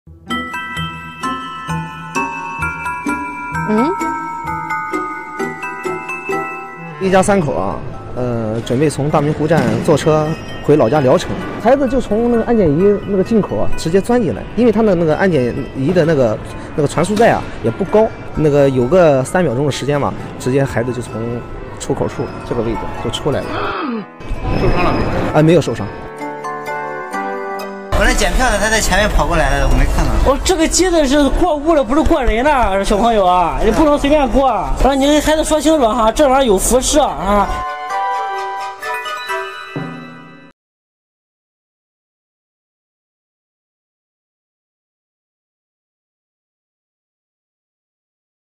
Moment five-year-old girl climbs into baggage X-ray machine
Security staff at a railway station in east China were shocked to see a silhouette of a human being on their monitors during a baggage inspection. A five-year-old girl climbed into the X-ray machine when her parents were not paying attention at Daminghu Railway Station in Jinan, Shandong province on Monday, Chinese media reported. Three seconds later, the girl appeared from the other side of the  machine and stepped off the conveyor belt in front of stunned officers.